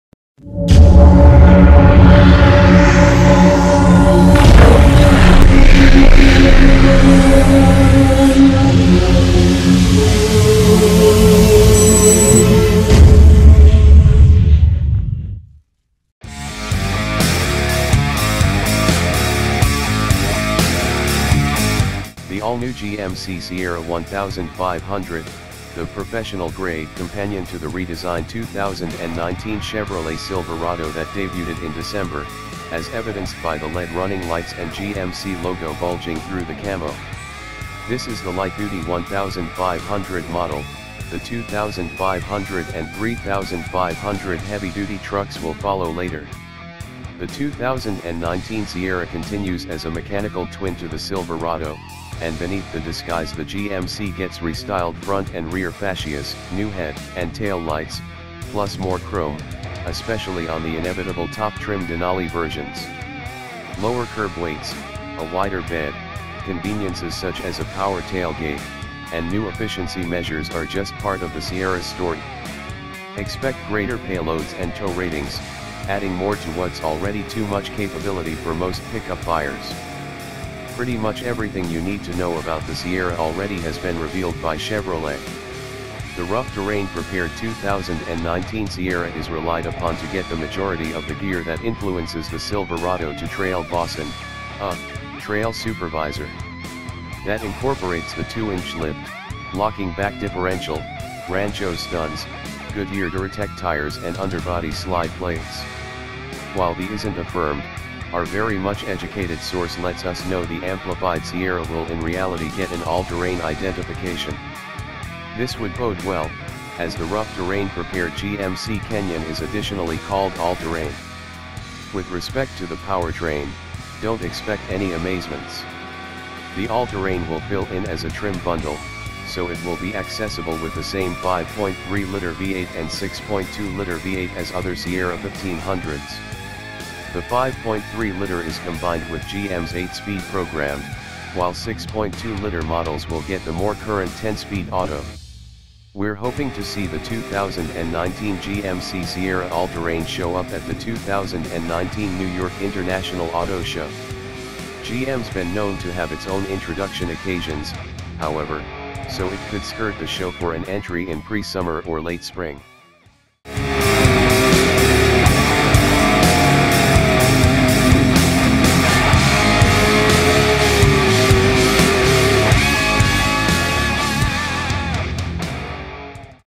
READY to FIGHT Ford F 150 !! - (2019) GMC Sierra
GMC is taking its most prominent pickup truck to the following level with the totally updated 2019 GMC Sierra.

Recognized by intense outline and section driving innovations, the cutting edge Sierra Denali by and by rethinks the exceptional pickup, while propelling Denali's heritage as the zenith of GMC plan, capacity and refinement.

It's all about cool cars
Hope U enjoy it